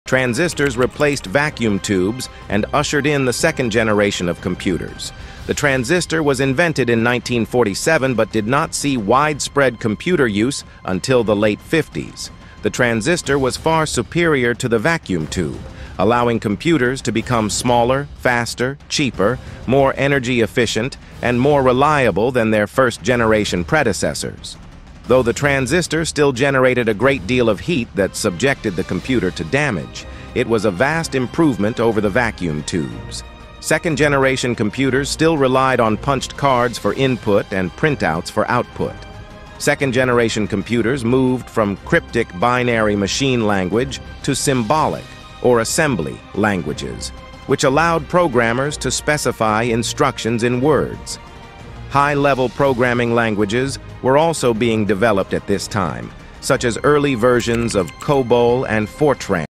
Second Generation Of Computers
The computer is such an important component of modern life that it is hard to imagine the world without it. In this video, you will learn the full history and evolution of computers to date.